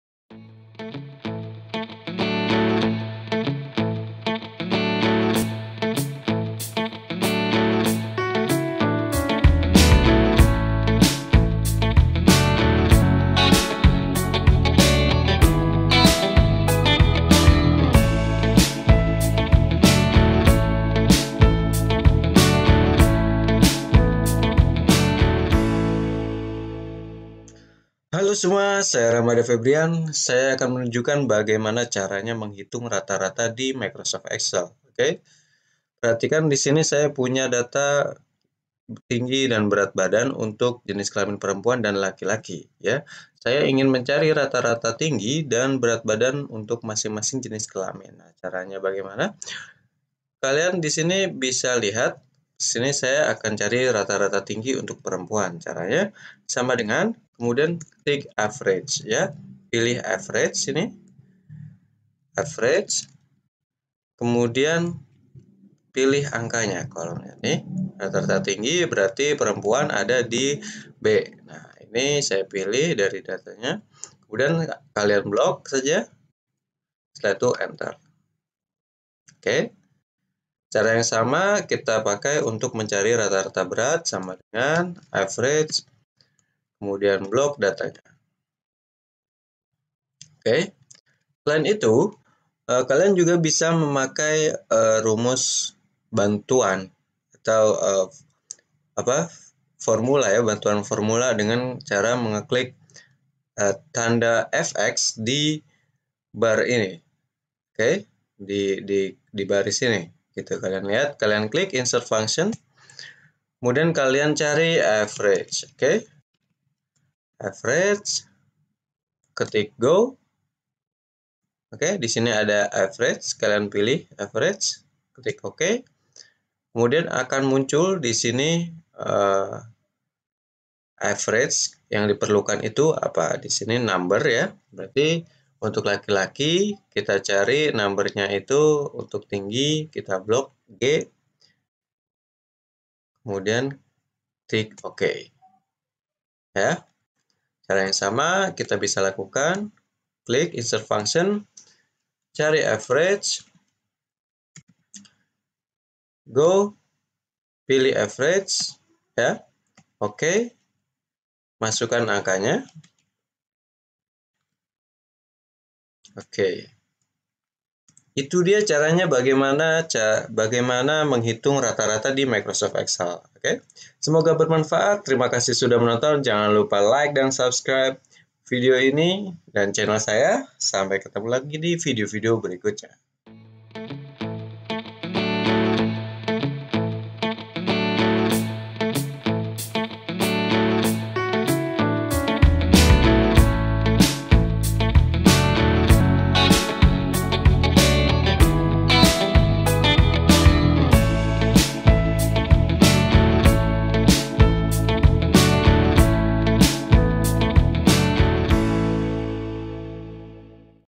Cara Menghitung Rata-rata Di Excel
Dalam video ini saya menggunakan Office 365, akan tetapi ini dapat juga dilakukan pada versi yang lebih lama.

👀 Tonton Playlist dan Video Terkait!